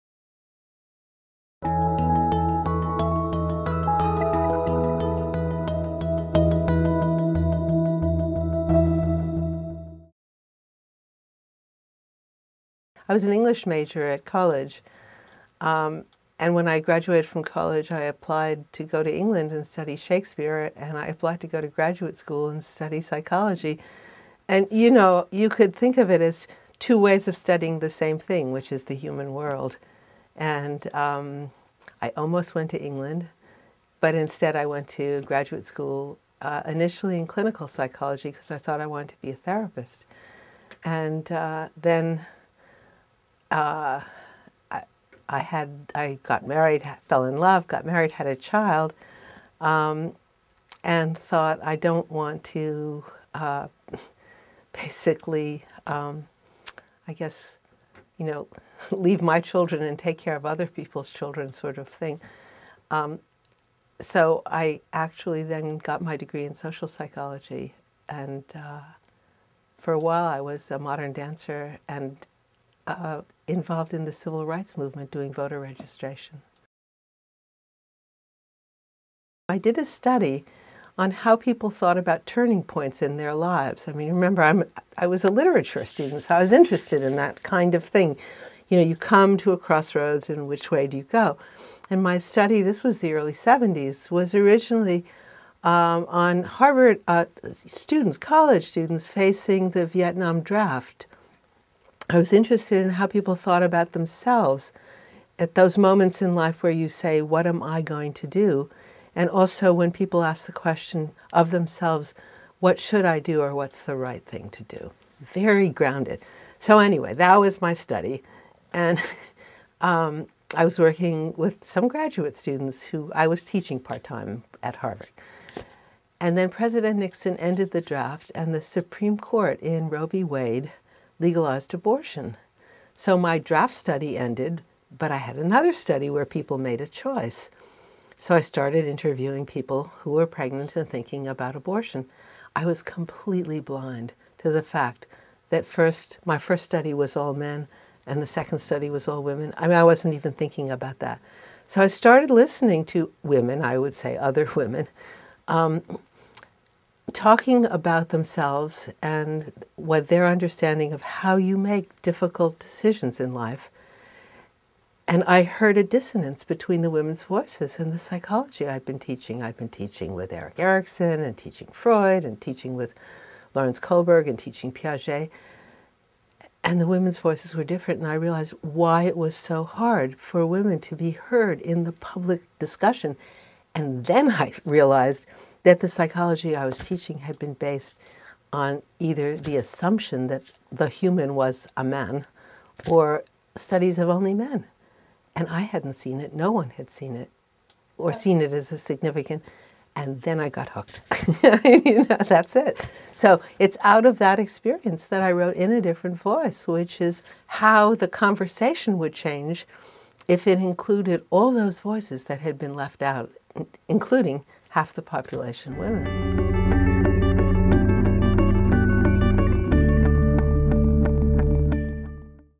Carol Gilligan on Becoming a Psychologist | Big Think
Carol Gilligan on Becoming a Psychologist
Carol Gilligan discusses her transition from literature to psychology.
Carol Gilligan:

In 2002, Carol Gilligan became University Professor at New York University, with affiliations in the School of Law, the Steinhardt School of Culture, Education, and Human Development, and the Graduate School of Arts and Sciences. She is currently teaching a seminar at the Law School on Resisting Injustice and an advanced research seminar on The Listening Guide Method of Psychological Inquiry.

She received an A.B. in English literature from Swarthmore College, a masters degree in clinical psychology from Radcliffe College and a Ph.D. in social psychology from Harvard University. Her landmark book In A Different Voice (1982) is described by Harvard University Press as "the little book that started a revolution."  Following In A Different Voice, she initiated the Harvard Project on Women's Psychology and Girls' Development and co-authored or edited 5 books with her students.

She received a Senior Research Scholar award from the Spencer Foundation, a Grawemeyer Award for her contributions to education, a Heinz Award for her contributions to understanding the human condition and was named by Time Magazine as one of the 25 most influential Americans.

She was a member of the Harvard faculty for over 30 years and in 1997 became Harvard's first professor of Gender Studies, occupying the Patricia Albjerg Graham chair.
TRANSCRIPT:

Question: How did you first get into psychology?

Carol Gilligan: I was in English major at college and when I graduated from college, I applied to go to England and study Shakespeare and I applied to go to graduate school and study psychology and you could think of it as two ways of studying the same thing, which is the human world and I almost went to England, but instead I went to graduate school, initially in clinical Psychology, because I thought I want to be therapist. Then I got married, fell in love, got married had a child and thought I don’t want to basically, I guess leave my children and take care of other people children sort of thing. So, I actually then got my degree in social psychology and for a while I was a modern dancer and involved in the civil rights movement doing voter registration.

Question: What was your first study?

Carol Gilligan: I did a study on how people thought about turning points in there lives, I mean remember I was at literature student, so I was interested in that kind of thing. You come to a cross roads in which way do you go, and my study this was the early 70s was originally on “Harvard Students College”, students facing the Vietnam draft. I was interested in how people thought about themselves at those moments in life where you say, “What am I going to do?” and also when people ask question of themselves “What should I do or what is the right thing to do?” Very grounded, so anyway that was my study and I was working with some graduate students who I was teaching part time at Harvard. And then President Nixon ended the draft and the Supreme Court in Roby Wade legalized abortion. So, my draft study ended, but I had another study where people made a choice. So, I started interviewing people who are pregnant and thinking about abortion. I was completely bond to the fact that my first study was all man and second study was all women, I mean wasn’t even thinking about that. So, I started listening to women I would say other women, talking about themselves and what their understanding of how you make difficult decisions in life and I heard a descendants between the women’s voices in psychology, I had been teaching, I have been teaching with there Erickson and teaching Ford and teaching with Lions Cobber, I am teaching Peache [phonetic] and the women’s voices where different and then I realized why it was so hard for a women to be heard in the public discussion and then I have realized that the Psychology I was teaching have been based on either of the assumption that human was a man or studies have only man, and I hadn’t seen it, no one had seen it, what seen it is significant and then I got hooked. I knew that is it. So, it is out of that experience that I really in a different force, which is how the conversation would change if it included all those voices that had been left out including half the population of women.